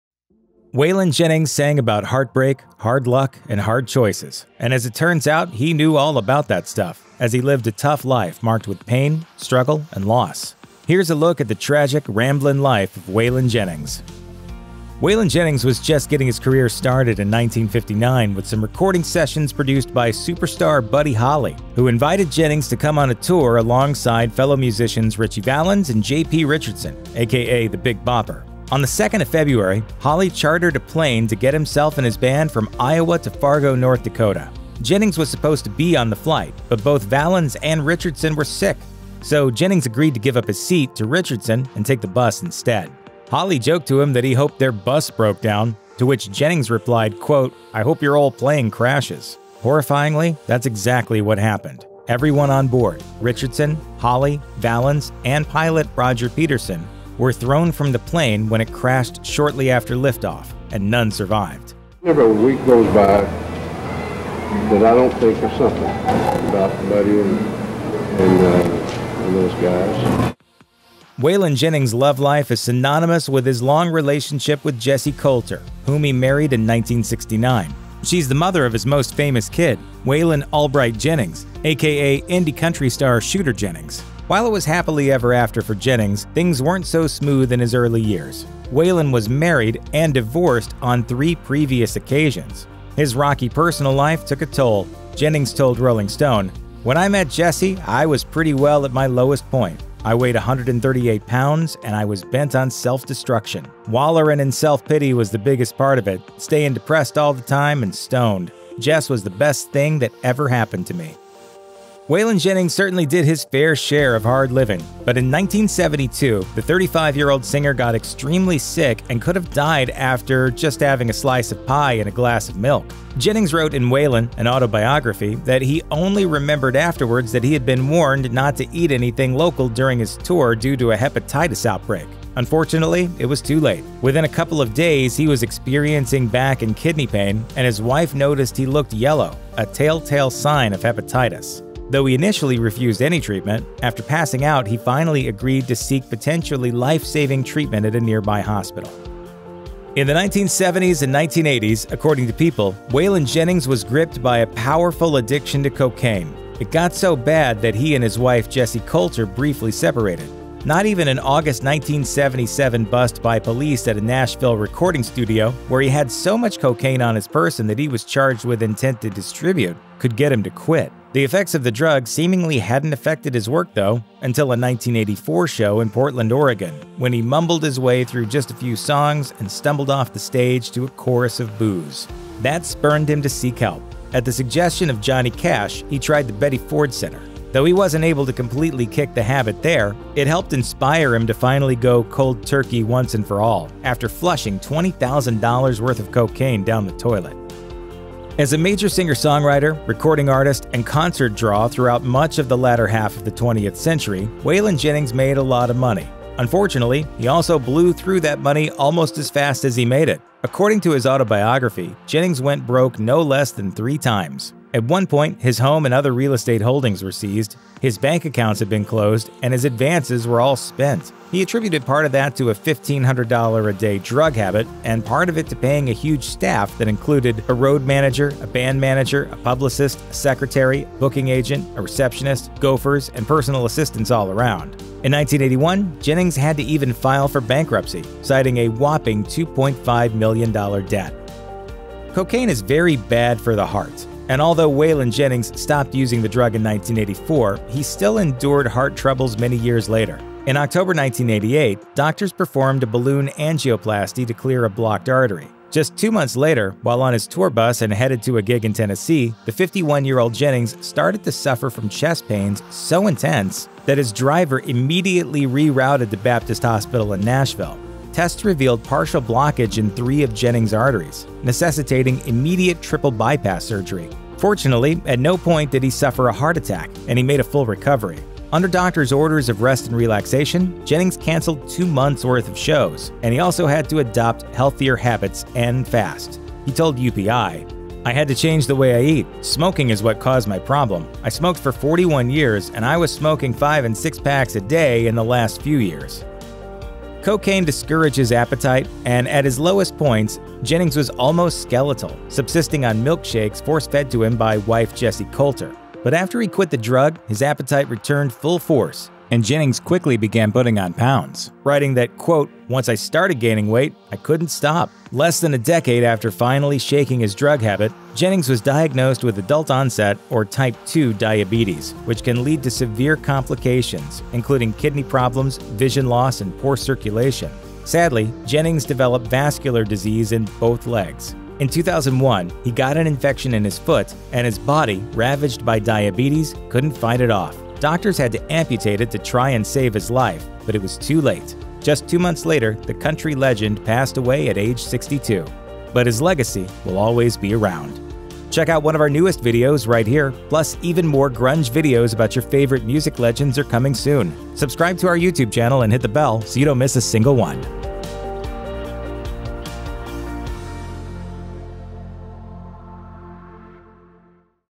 Tragic Details About Waylon Jennings
Waylon Jennings was one of the bad boys of country music, and that image came from his real life just as much as his stage persona and music. The outlaw country singer had quite a tragic life from his earliest days in the spotlight, when a young Jennings gave up his seat on a plane to J.P. “The Big Bopper” Richardson. The plane, which also carried Buddy Holly and Richie Valens, crashed and killed everyone on board. The near-death experience haunted Jennings for the rest of his life. From his failed marriages to his drug problems, let’s take a look at some tragic details about Waylon Jennings.

The day the music died | 0:00
The basics of love | 1:10
Hepatitis scare | 1:52
Cocaine nearly killed him | 2:29
Bankruptcy woes | 3:17
Heart problems | 4:02
A sad end for a legendary Highwayman | 4:56